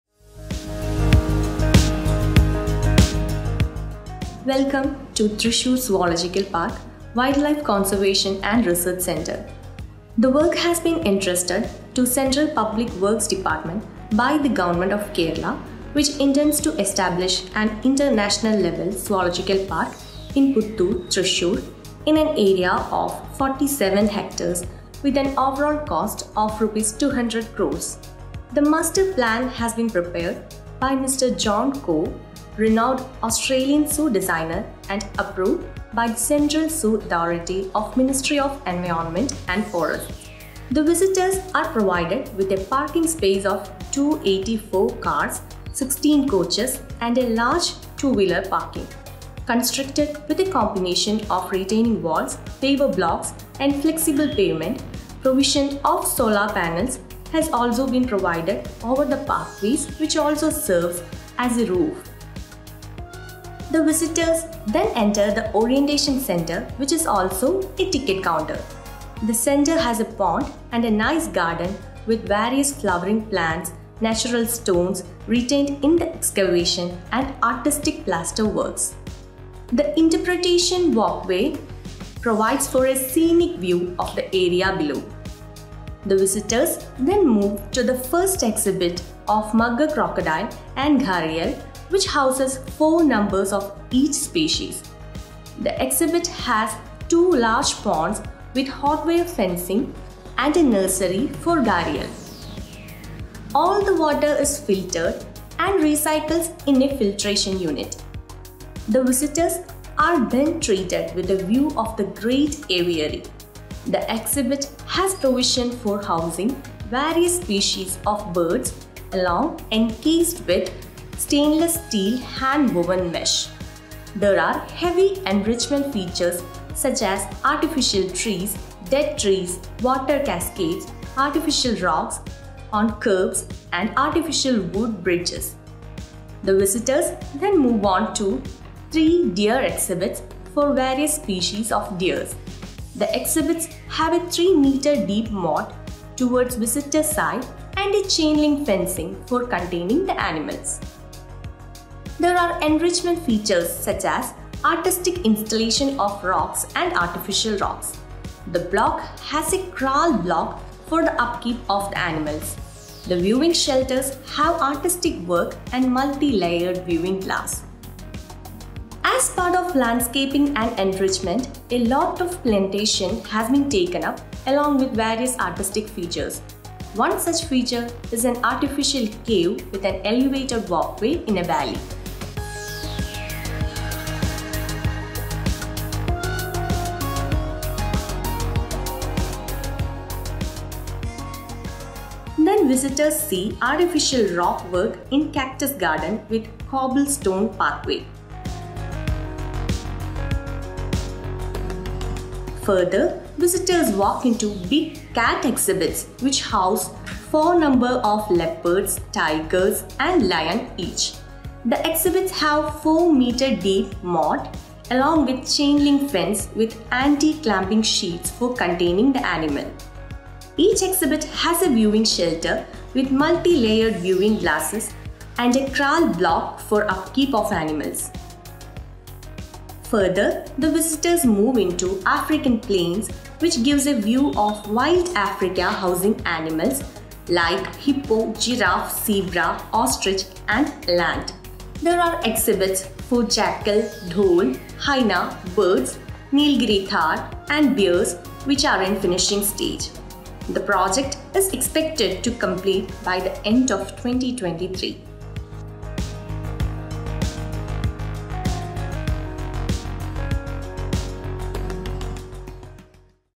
Thrissur Zoological Park Puthur | HD Drone View | Largest In Asia
Credits: Executive Engineer, TPD,
CPWD- Shri. Shashwat Gaur

Commentary : Smt. Swathi B Raj

Video : Sopanam Wedding Company, Puthur